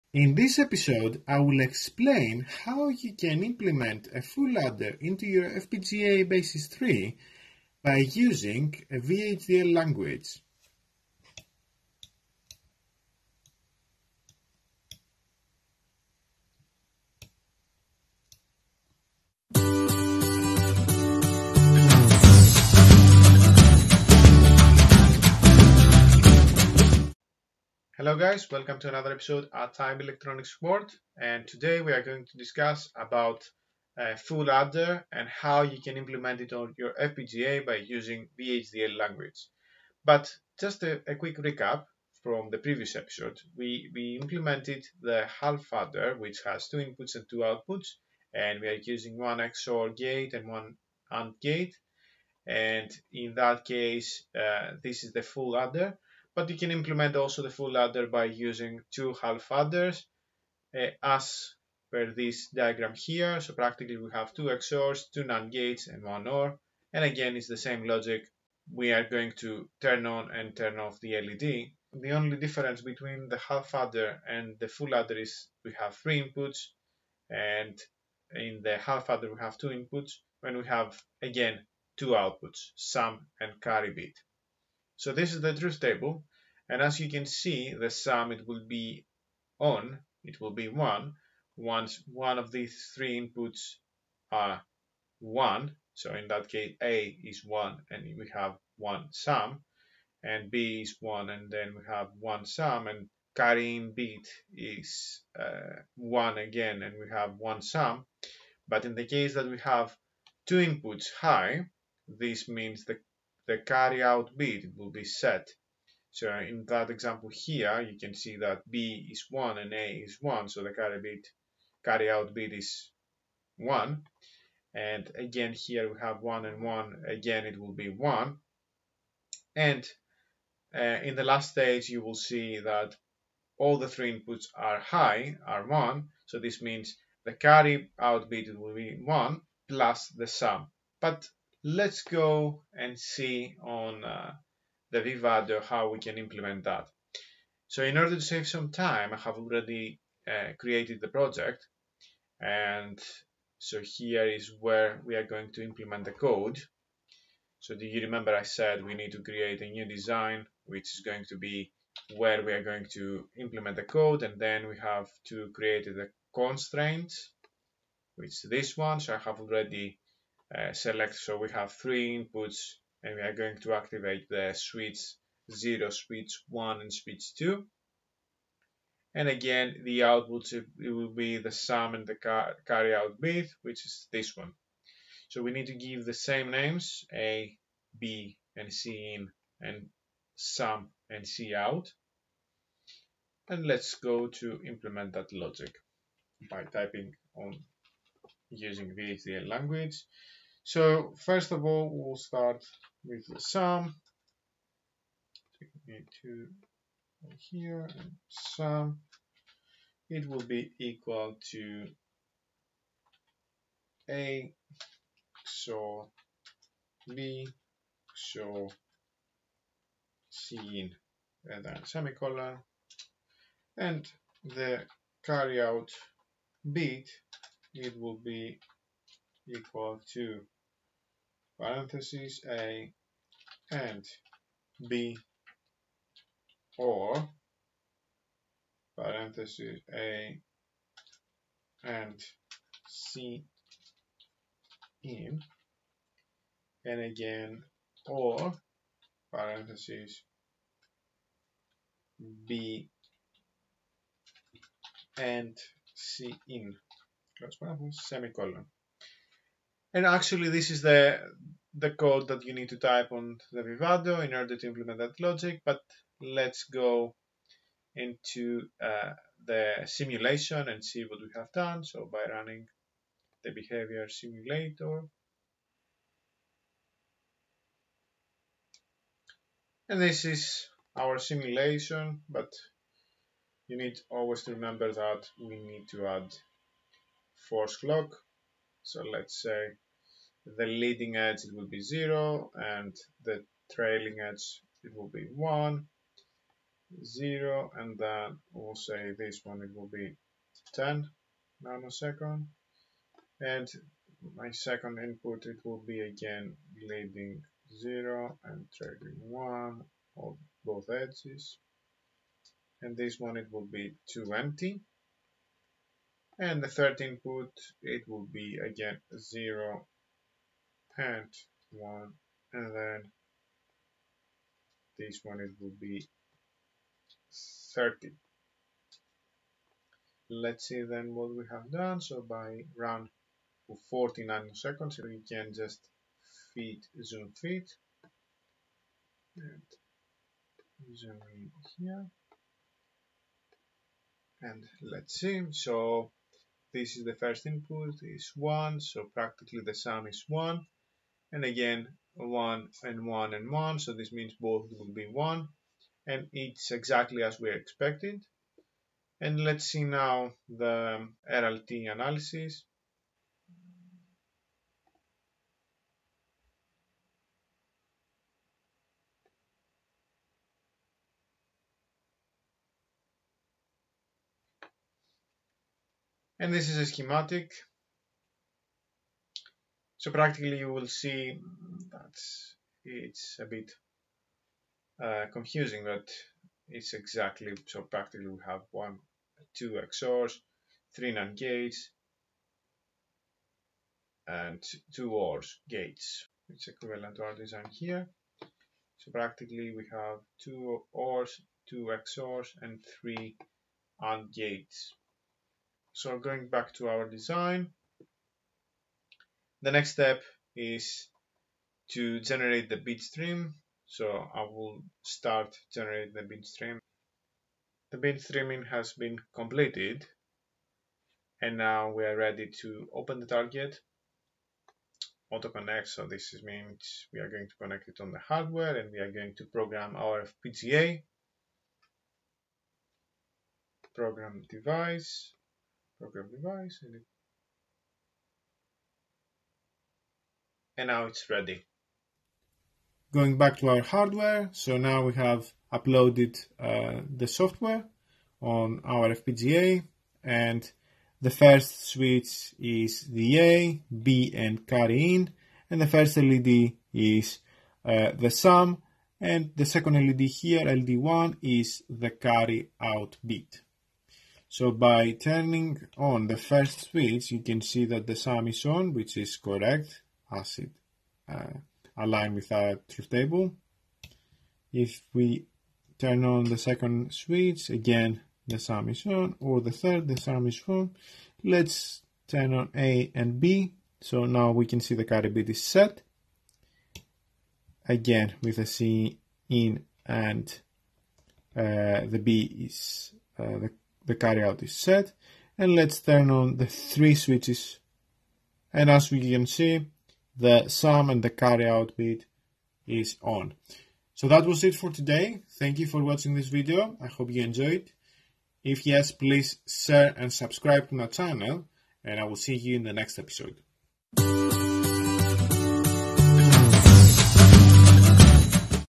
How to build a Full Adder on your FPGA(VHDL).
This is a tutorial that explains how you can build a Full_Adder by using FPGA Basys3 and VHDL language.